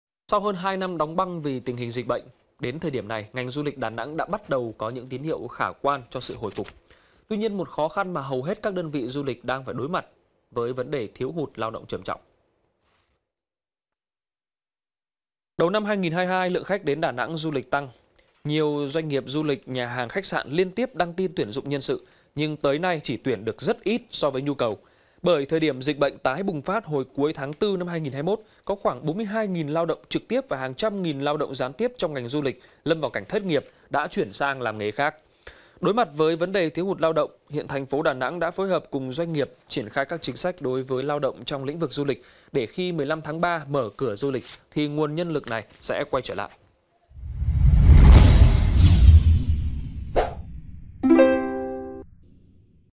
Nhiều người tố giác lừa đặt phòng khách sạn ở Đà Lạt| VTC14
VTC14 |Nhiều du khách phản ánh, đặt phòng khách sạn qua mạng xã hội khi muốn đi Đà Lạt du lịch đã bị lừa tiền cọc lên đến hàng chục triệu đồng. Hiện cơ quan công an cũng đã nhận được đơn tố cáo của các nạn nhân.